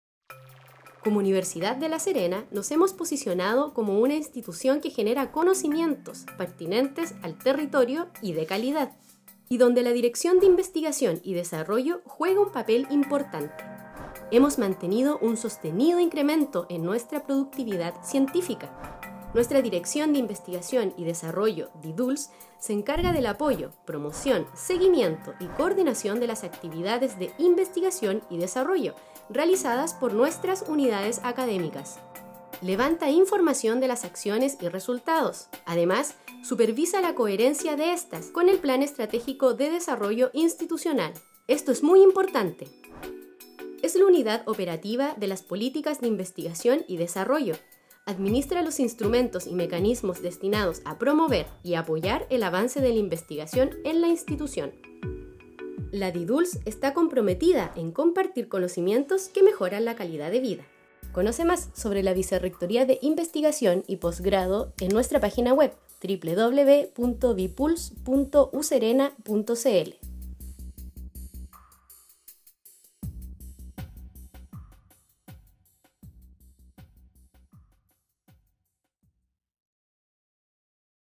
Conoce la VIPULS | Dirección de Investigación y Desarrollo
La Dirección de Investigación y Desarrollo, de la Universidad de La Serena se encarga del apoyo, promoción seguimiento y coordinación de las actividades de Investigación y Desarrollo, realizadas por nuestras unidades académicas.